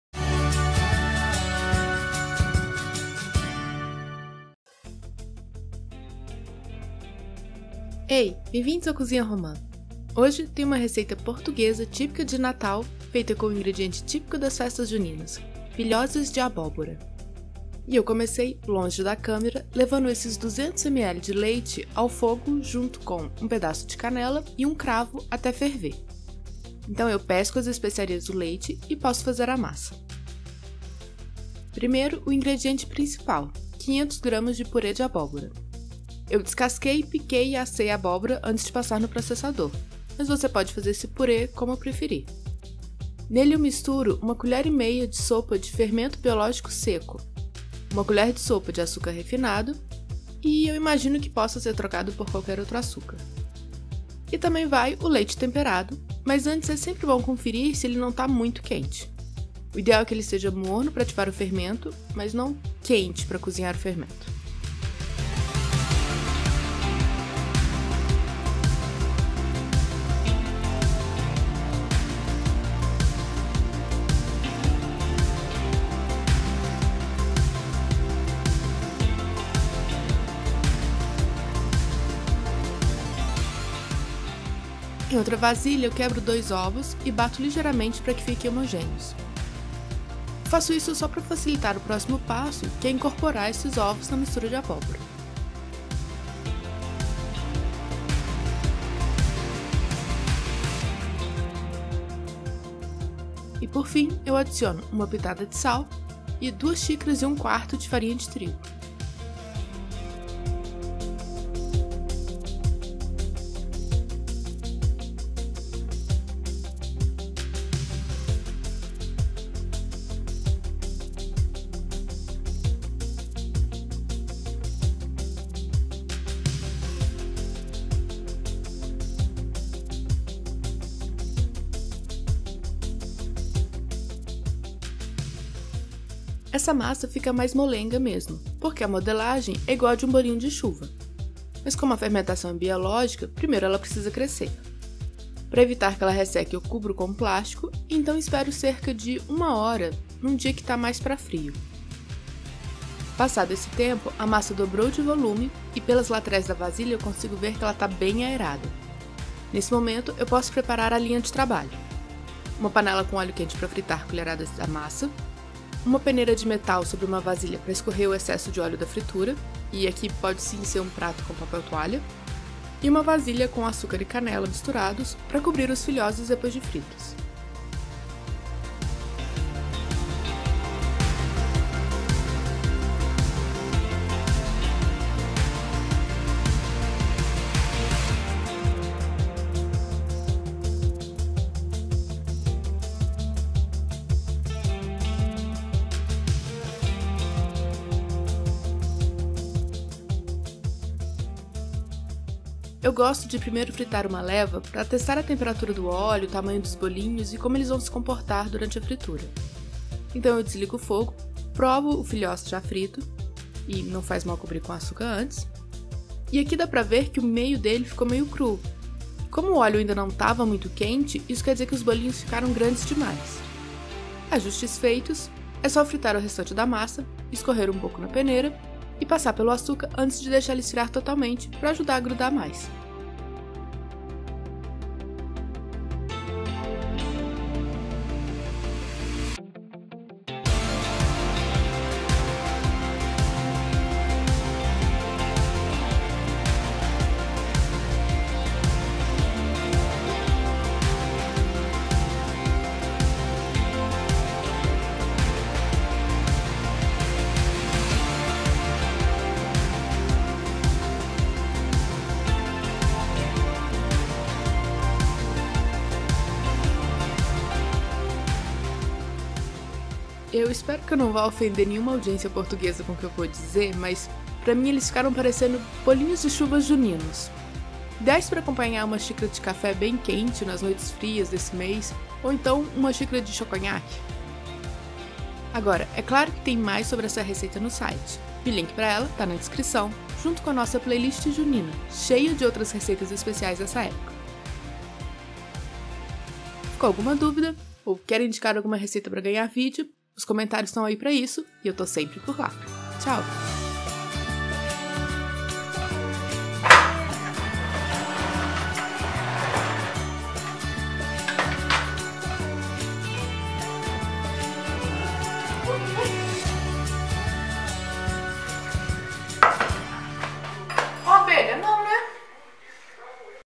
Filhoses de abóbora
Filhoses são os primos portugueses dos nossos bolinhos de chuva... será?

Feitos com purê de abóbora como base e com fermentação biológica, esses bolinhos fritos e cobertos de açúcar fazem bem o papel de acompanhante de uma xícara de café numa noite fria. Como as noites juninas!

Quer ver a receita completa por escrito? O link é esse

E claro, pra tirar qualquer dúvida é só chamar nos comentários!

E não esqueça de acompanhar o CozinhaRoman em outras redes!

 Vídeo editado por BartRoman